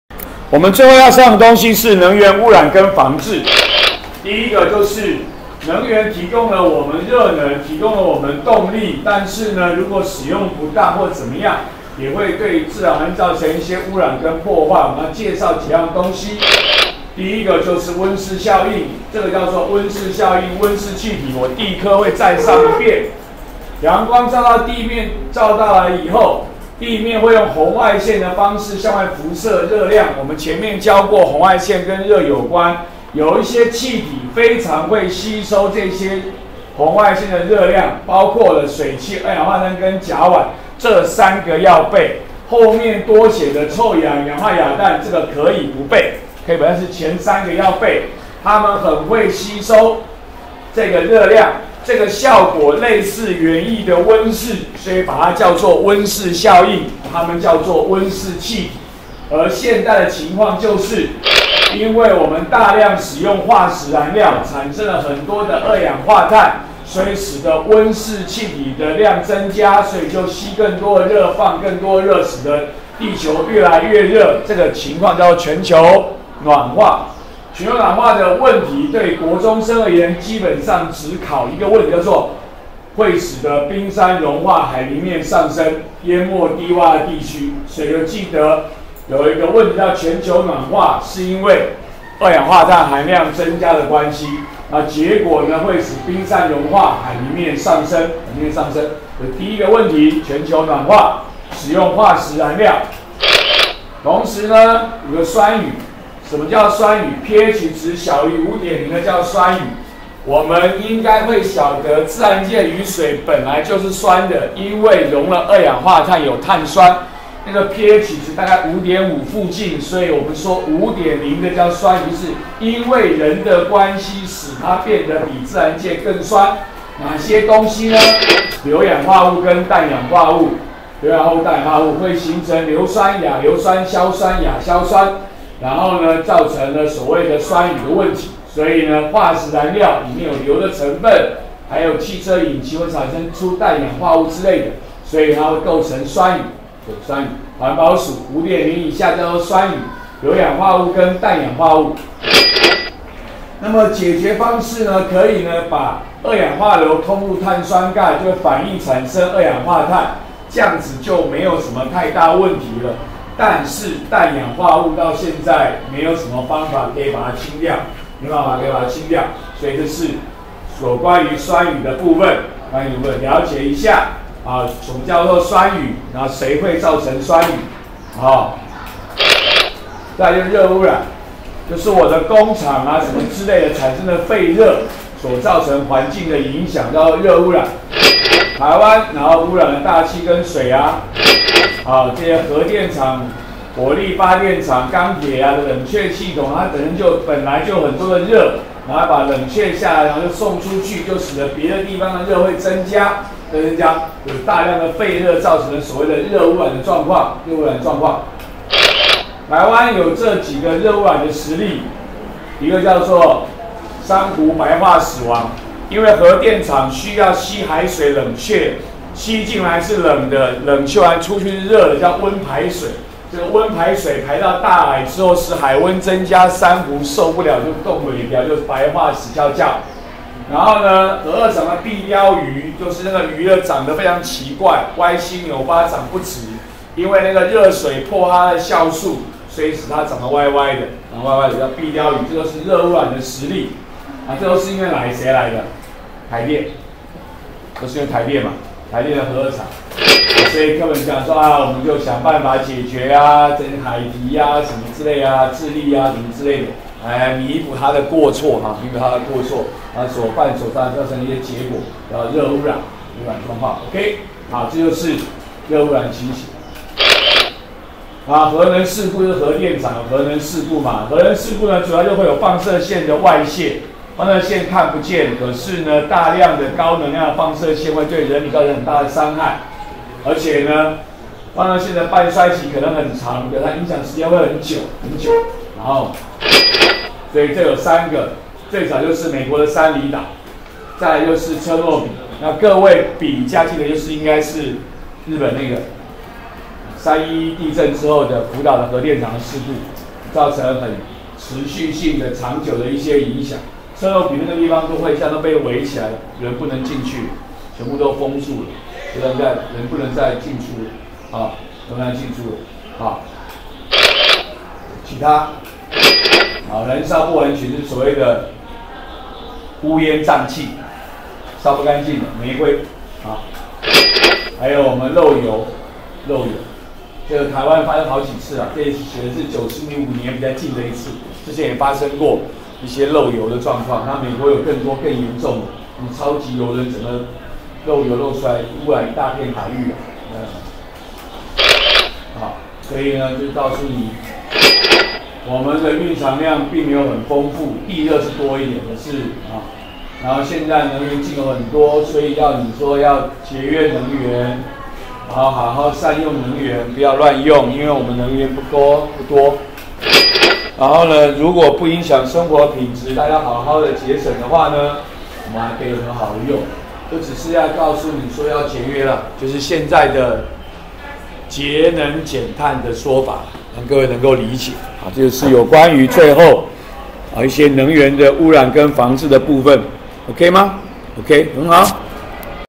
103學年國三理化 能源11：能源汙染與防制
介紹全球暖化、酸雨、熱汙染、核能事故，煤灰及漏油，最後提及節約能源。